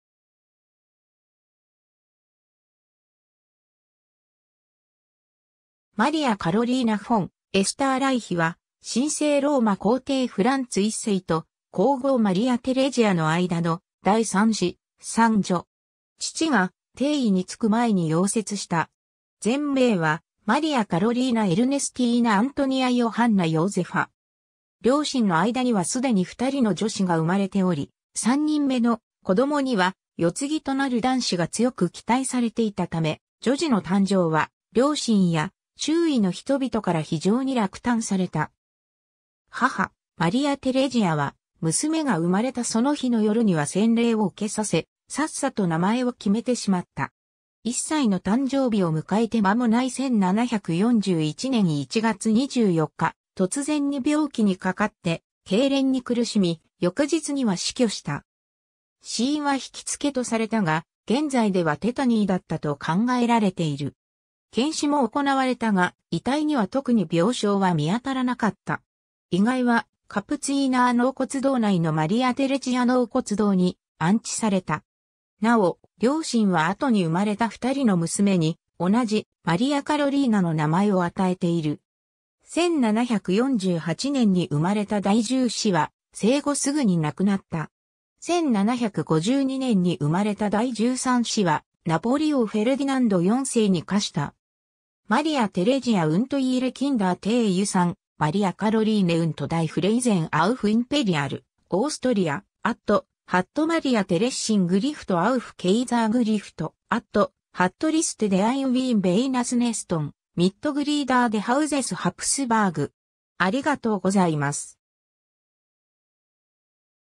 マリア・カロリーナ・フォン・エスターライヒ (1740-1741)
フランツ1世とマリア・テレジアの子女
ハプスブルク＝ロートリンゲン家
マリア・カロリーナ・フォン・エスターライヒ（Maria Karolina von Österreich, 1740年1月12日 - 1741年1月25日）は、神聖ローマ皇帝フランツ1世と皇后マリア・テレジアの間の第3子、三女。
父が帝位に就く前に夭折した。
全名はマリア・カロリーナ・エルネスティーナ・アントニア・ヨハンナ・ヨーゼファ（Maria Karolina Ernestina Antonia Johanna Josepha）。
 両親の間にはすでに2人の女子が生まれており、3人目の子供には世継ぎとなる男子が強く期待されていたため、女児の誕生は両親や周囲の人々から非常に落胆された。
母マリア・テレジアは娘が生まれたその日の夜には洗礼を受けさせ、さっさと名前を決めてしまった。
 1歳の誕生日を迎えて間もない1741年1月24日、突然に病気に罹って痙攣に苦しみ、翌日には死去した。
死因はひきつけとされたが、現在ではテタニーだったと考えられている。
検死も行われたが、遺体には特に病症は見当たらなかった。
遺骸はカプツィーナー納骨堂内のマリア・テレジア納骨堂（Maria-Theresien Gruft）に安置された。
 なお、両親は後に生まれた2人の娘に同じ「マリア・カロリーナ」の名前を与えている。
1748年に生まれた第10子（七女）は生後すぐに亡くなった。
1752年に生まれた第13子（十女）は、ナポリ王フェルディナンド4世に嫁した。
 ^ Maria Theresia und ihre Kinder - Teil 3: Maria Karoline und die Fraisen auf imperial-austria.at ^ Maria Theresien Gruft auf Kaisergruft.at ^ Liste der in Wien beigesetzten Mitglieder des Hauses Habsburg